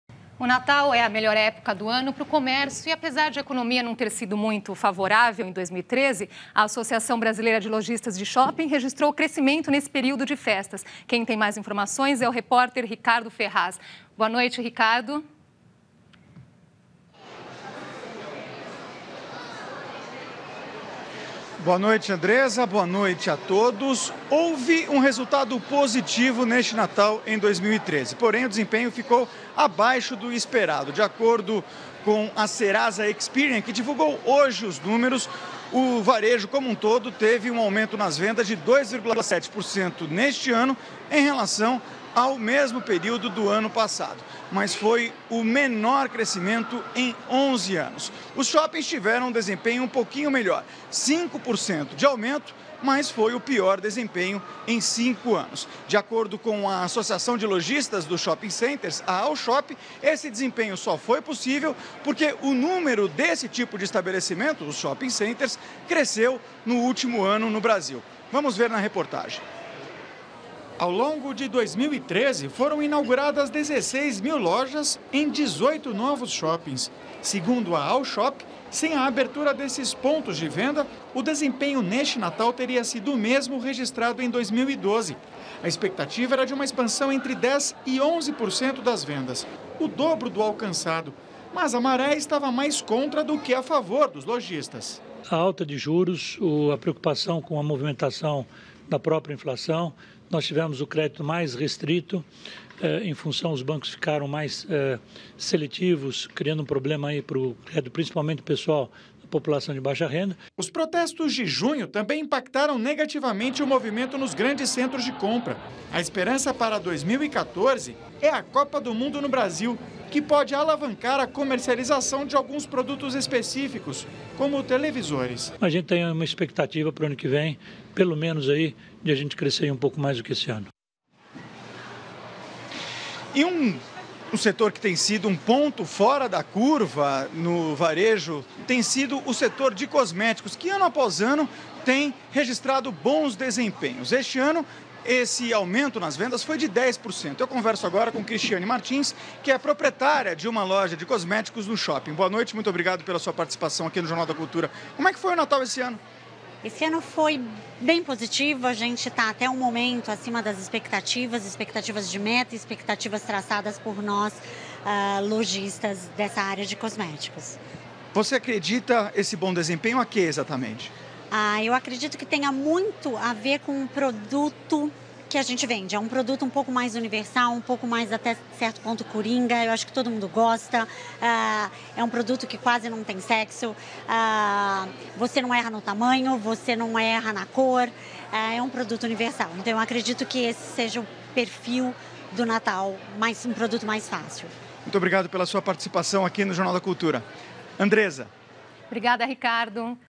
Associação Brasileira de Lojistas registra crescimento em compras nesse período de festas
A expectativa era de uma expansão entre 10 e 11% das vendas, o dobro do alcançado. Mas a maré estava mais contra do que a favor dos lojistas